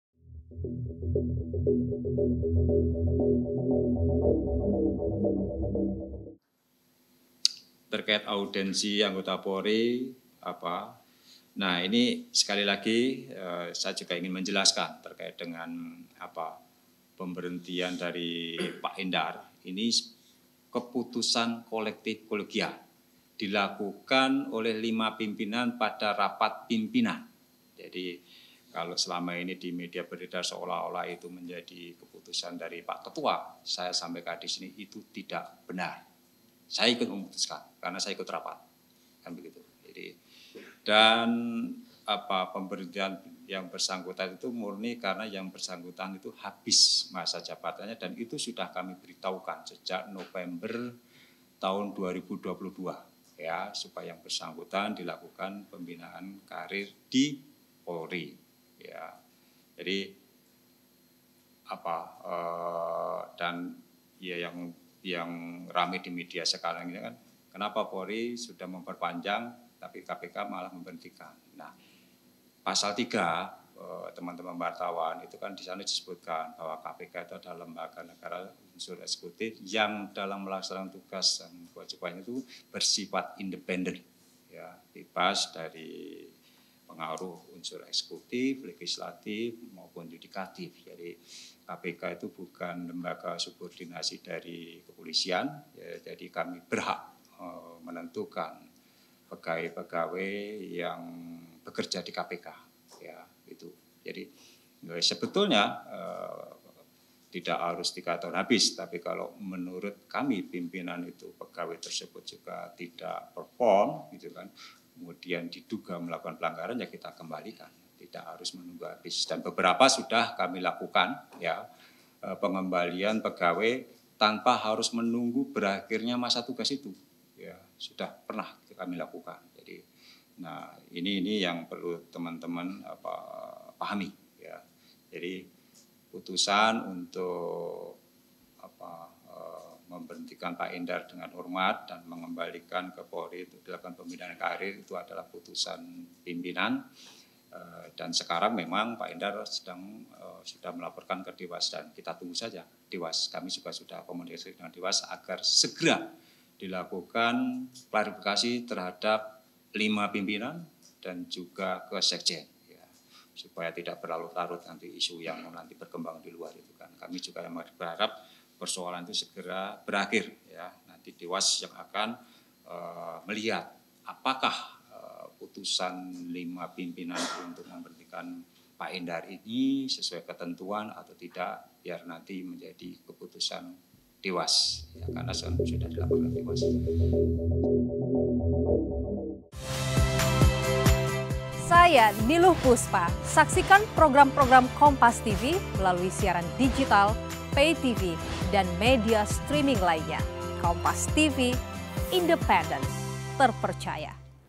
Soal Polemik Brigjen Endar, KPK Tegaskan Bukan Lembaga Bawahan Polri
JAKARTA, KOMPAS.TV - Wakil Ketua KPK Alexander Marwata menegaskan bahwa KPK bukan lembaga subordinasi atau bawahan Polri. Menurut Alex, hal itu membuat pihaknya berhak menentukan pegawai yang bekerja di KPK.

Pernyataan ini Alex sampaikan untuk menanggapi sejumlah pihak yang menyayangkan KPK memberhentikan Brigjen Endar Priantoro dari jabatan Direktur Penyelidikan.

Padahal, Kapolri Jenderal Listyo Sigit Prabowo telah memperpanjang masa penugasannya di KPK.

"Jadi, KPK itu bukan lembaga subordinasi dari kepolisian. Jadi kami berhak menentukan pegawai-pegawai yang bekerja di KPK," jelas Alex, Sabtu (8/4/2023).

Baca Juga Seluruh Pimpinan KPK Setujui Pencopotan Brigjen Endar, KPK: Masa Jabatan Brigjen Endar Sudah Selesai di